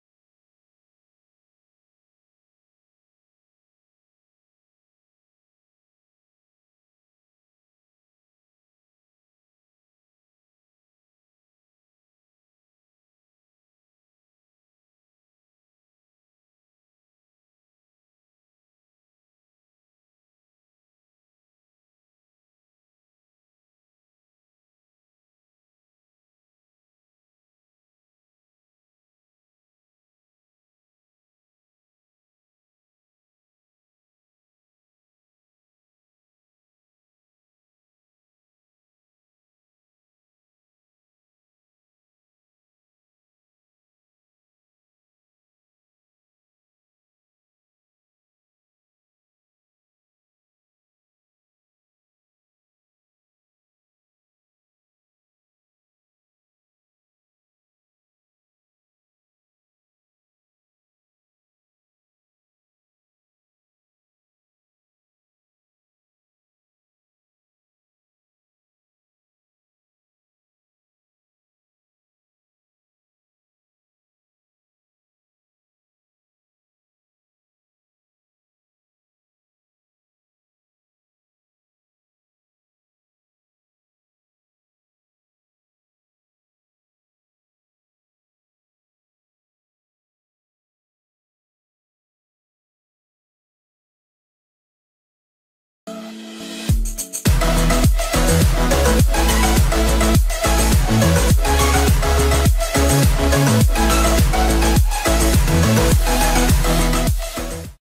How to change your Youtube Channel Name to 1 Word[2017]
Hey peeps! I already made a video on how to do this a year ago but i found out how to do it 2 different ways so here you guys go!